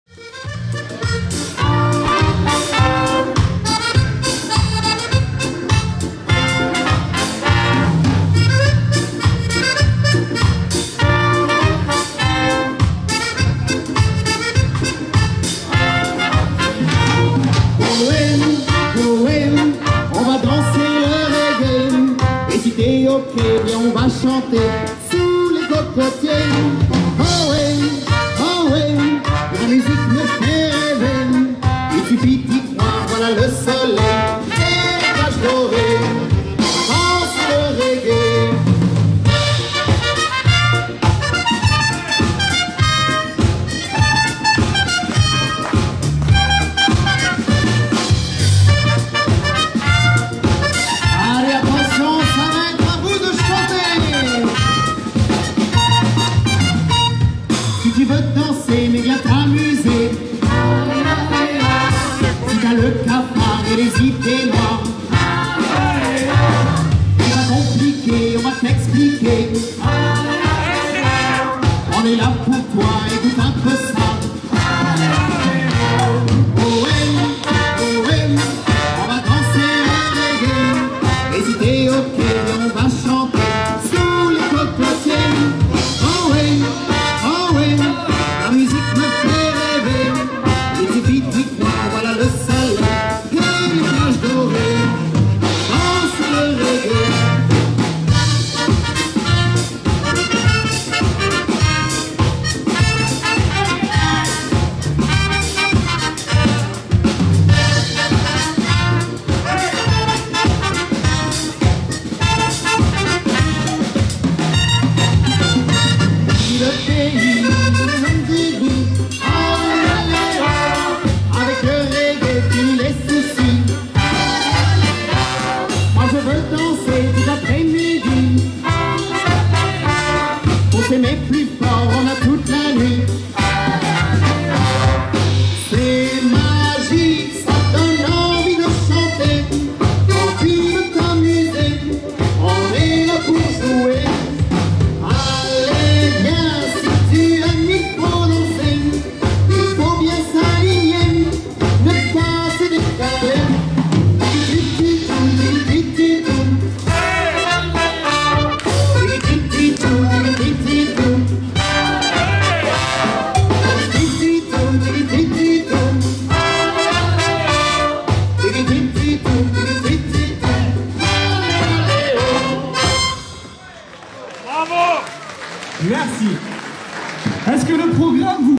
Céline COURNIL La TRONQUIERE oct 2015 Reggae
Gala d'accordéon LA TRONQUIERE (46) 3 octobre 2015/Orchestre Céline et Jérémy/Vidéo et réalisation Alexandra ASTORG (Sandy)